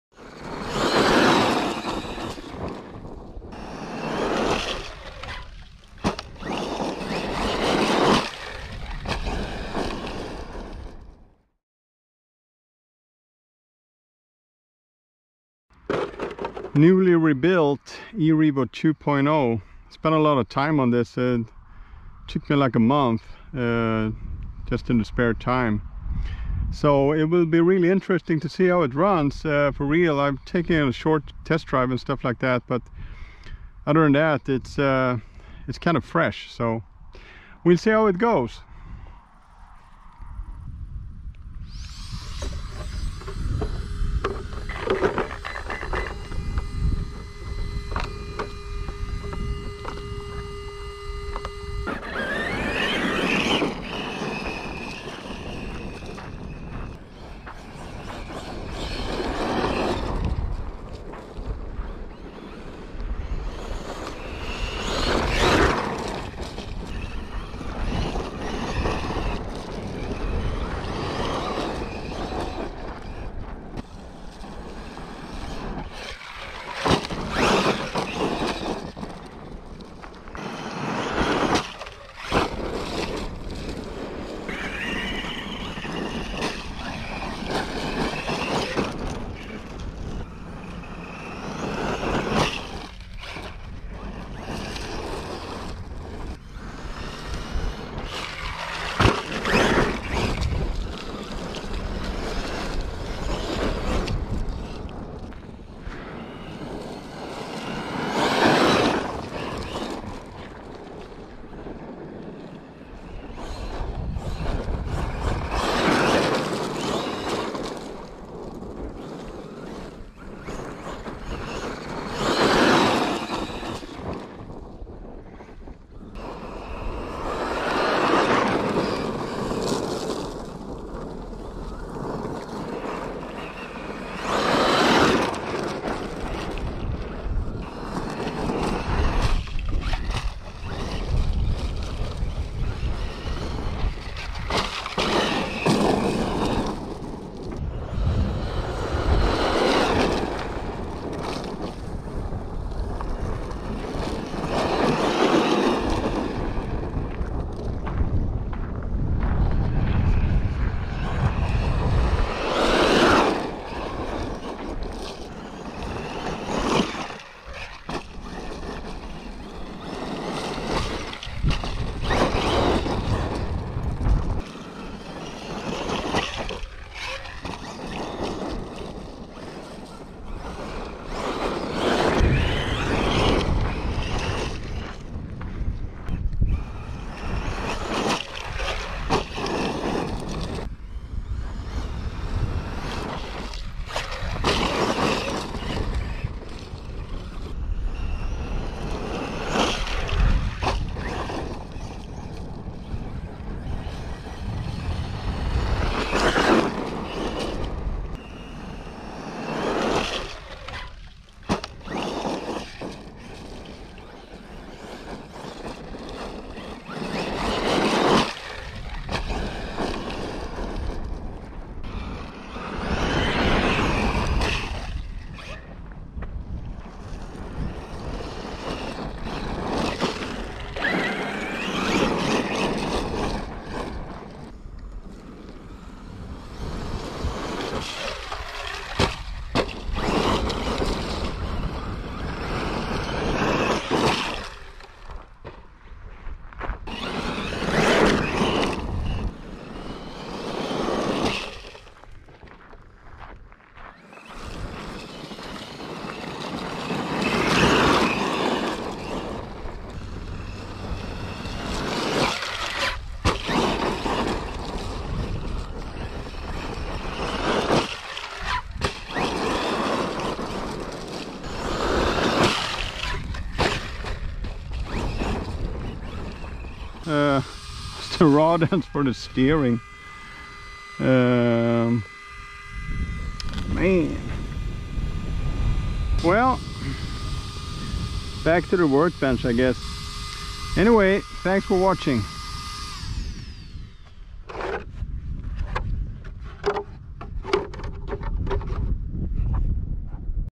Traxxas E-Revo 2.0 first bash after complete rebuild
needs some tuning :)